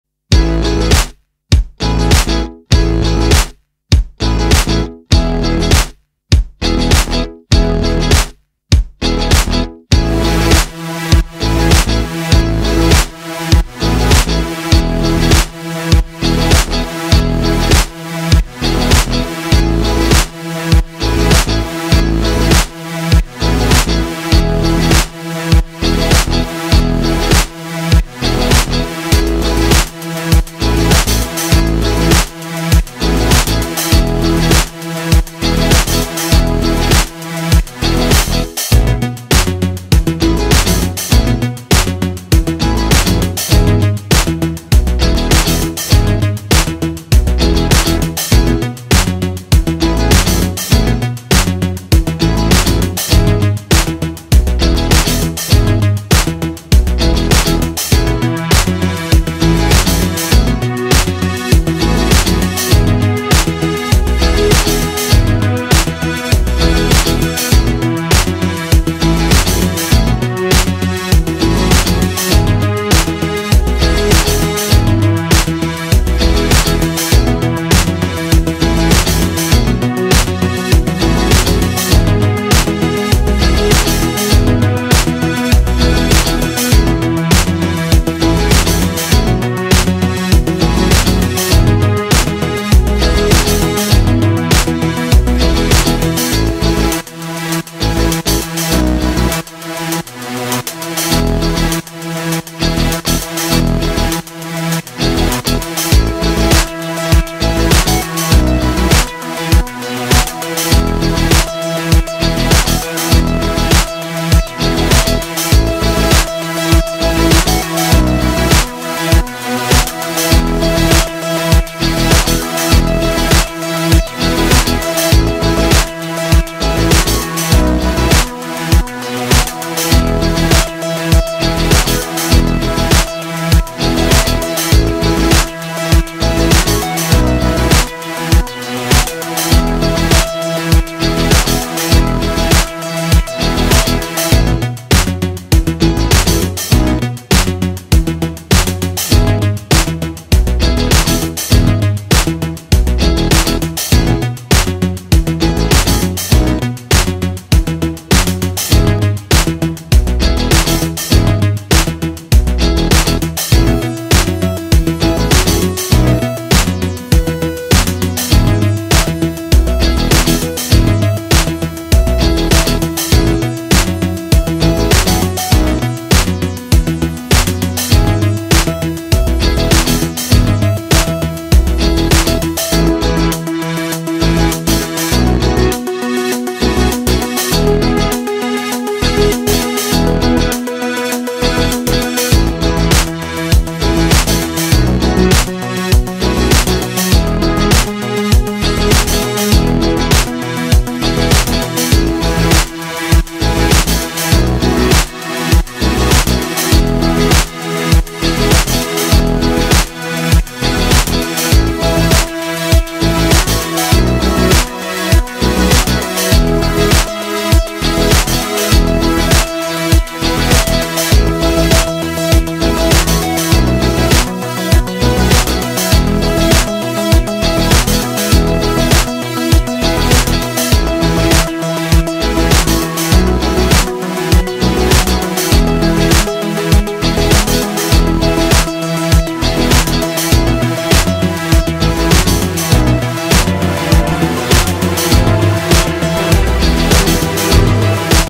An afternoon at Hull Paragon Interchange (3/7/21)
Welcome to this video about our trip to Hull Paragon Interchange by Humber Trains (Jacob) and Transport with Corey. In this video Jacob and Corey visit Hull Paragon Interchange. Enjoy!